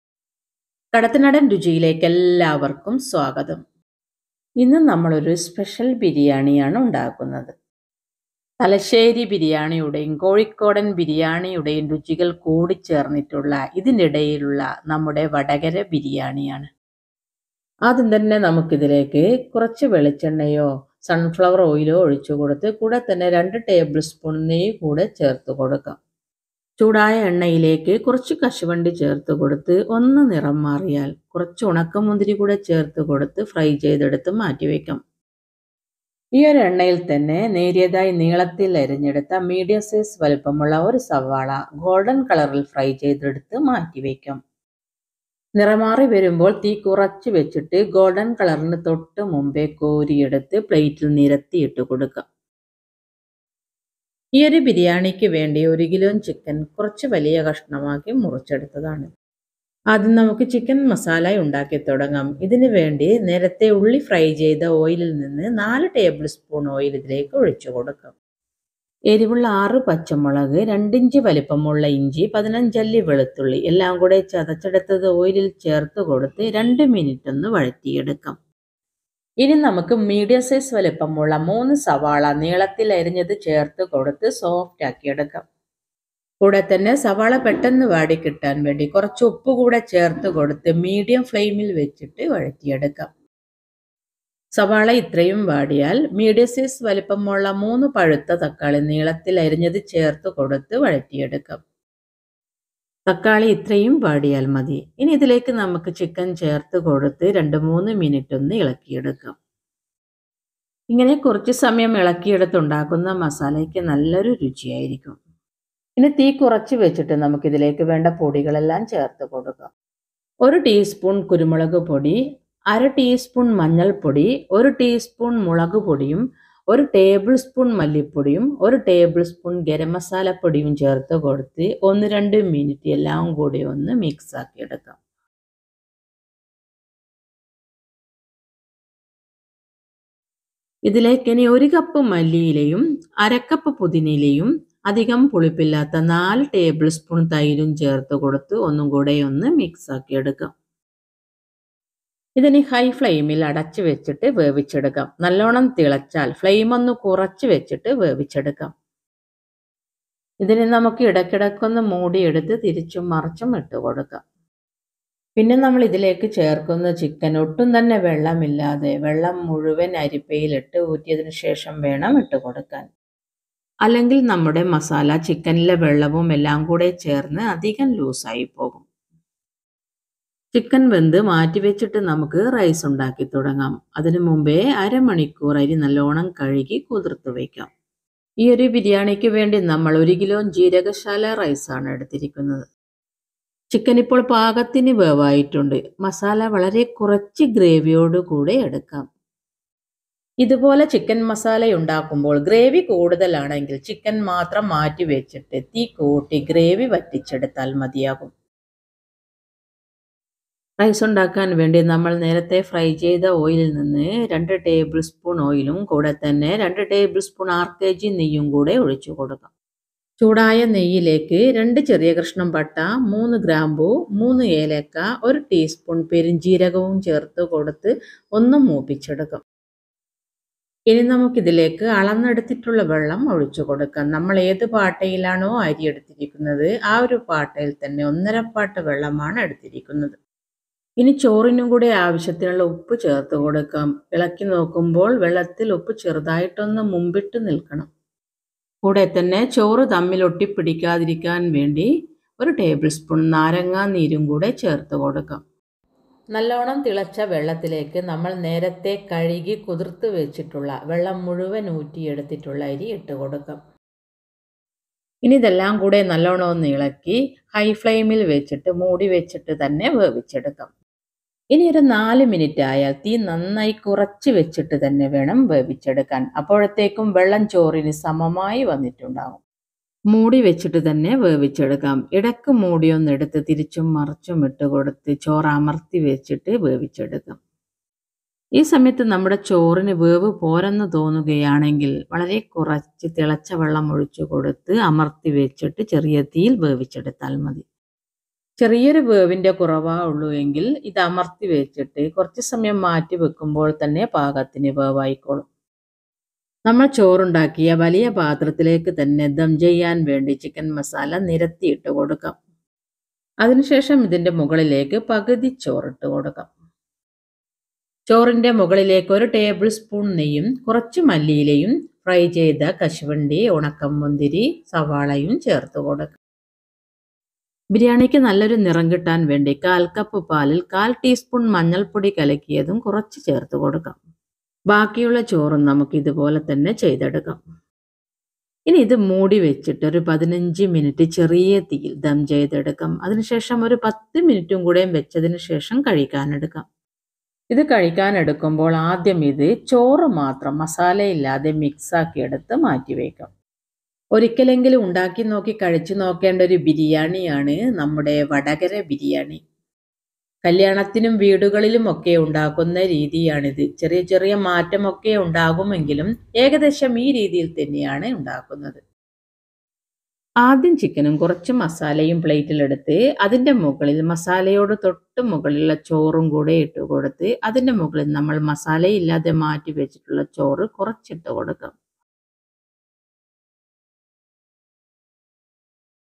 ഒരിക്കൽ എങ്കിലും ഉണ്ടാക്കി കഴിക്കേണ്ട ബിരിയാണി// Vadakara Kalyana biryani/ Kozhikoden biryani
എന്റെ പേര് ബിനില ഈയൊരു ബിരിയാണി, ബിരിയാണി ഇഷ്ടമുള്ളവർ ഒരിക്കൽ എങ്കിലും ഉണ്ടാക്കി കഴിക്കേണ്ട ഒരു ബിരിയാണിയാണ്   ജീരകശാല അഥവാ കൈമാറൈസ് കൊണ്ടുള്ള ബിരിയാണി
 തലശ്ശേരി ബിരിയാണിയുടെയും കോഴിക്കോടൻ ബിരിയാണിയുടെയും രുചികൾ കൂടിച്ചേർന്ന നല്ല നിറവും എരിവും ഒക്കെയുള്ള ബിരിയാണിയാണിത് തുടക്കക്കാർക്ക് പോലും ഒരു ബുദ്ധിമുട്ടുമില്ലാതെ വളരെ രുചികരമായി ഈ ഒരു ബിരിയാണി ഉണ്ടാക്കിയെടുക്കാൻ പറ്റും👌

 Jeera kashala rice          1k
 chicken                              1k
 water                                  1cup rice 11/2cup water
 oil
Salt
 Coriander leaves
 Tomato                              3Nos medium size
 Ginger                                2inch
 Garlic                                  15cloves
 Fennel seeds                      1tsp
Mint leaves                          1/4cup
 Coriander powder             1tbls
 Bllack pepper powder     1tsp
 Chilli powder                     1tsp
Lime juice                           1tbls
Cashew nuts
Raisins
 Garam Masala                 1tbls
Clove                                  3nos
 Cinnamon stick              2inch piece
Curd                                   4tbls
Chilli     6nos
Gardmon 4nos
Ghee

ചിക്കൻ കൊണ്ടുള്ള മലബാർ സ്റ്റൈൽ ഇറച്ചി ചോറ്

ഒറിജിനൽ കോഴിക്കോടൻ ചിക്കൻ ദം ബിരിയാണി

മലബാർ സ്റ്റൈൽ പൊരിച്ചചിക്കൻ ദം ബിരിയാണി